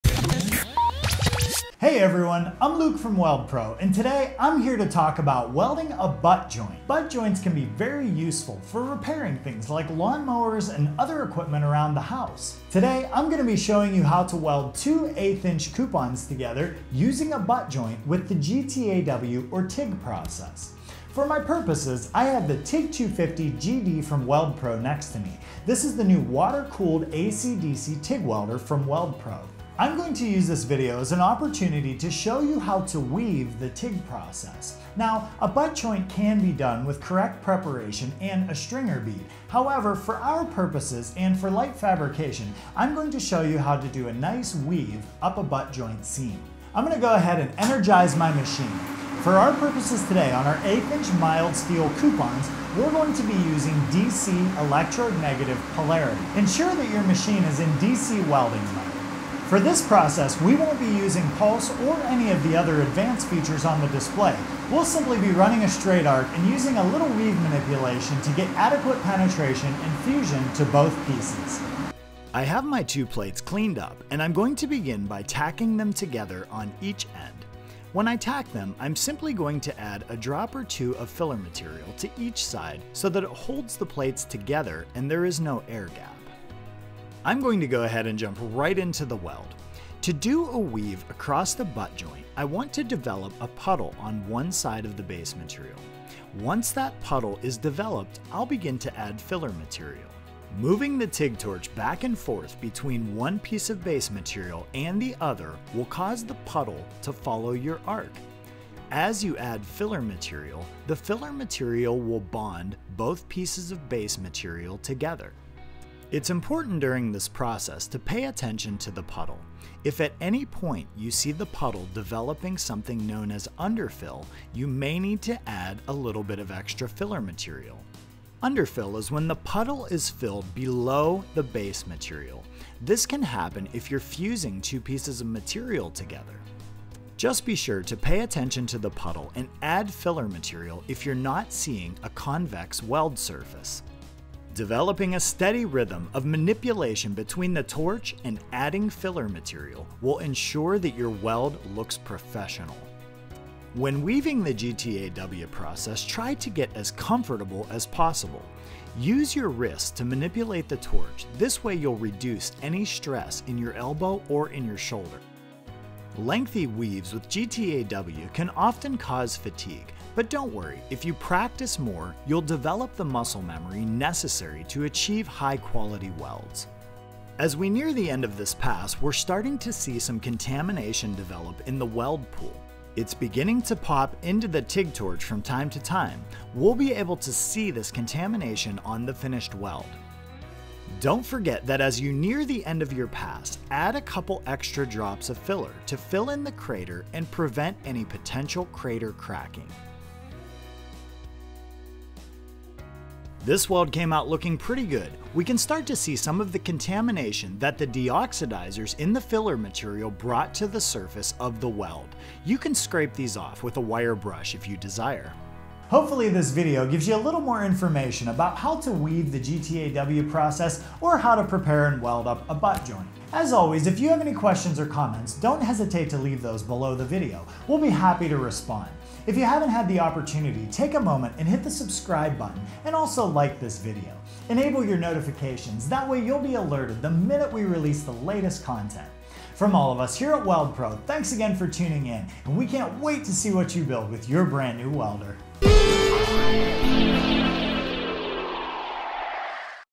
Weldpro TIG250 WEAVE GTAW
In this video we will discuss how to do a weave technique on mild steel with a simple butt joint as would be used in a household or farm repair situation. It is important to clean your base material well to reduce contamination as much as possible. If you haven't had a chance to check out the entire line of Weldpro TIG machines you can do so